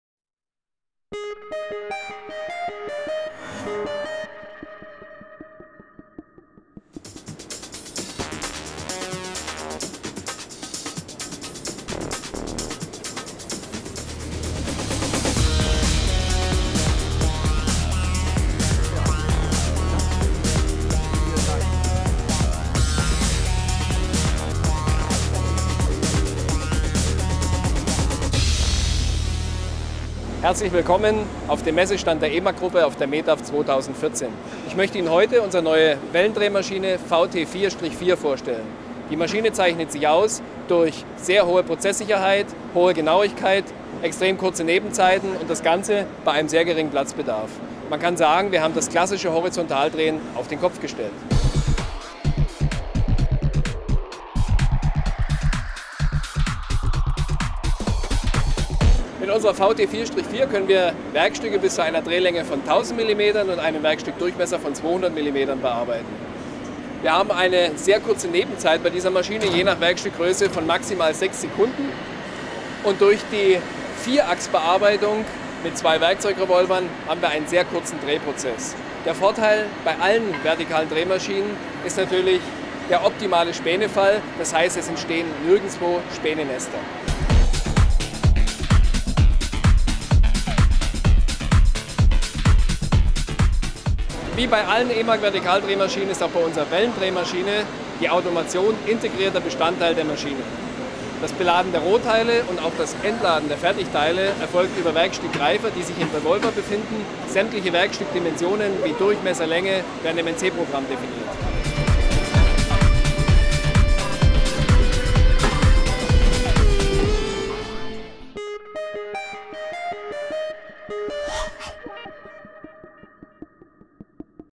EMAG Holding auf der METAV 2014 - CNC Arena TV
Falk Herkner, EMAG Vertriebsleiter Deutschland, stellt die vertikale Wellendrehmaschine VT 4-4 vor.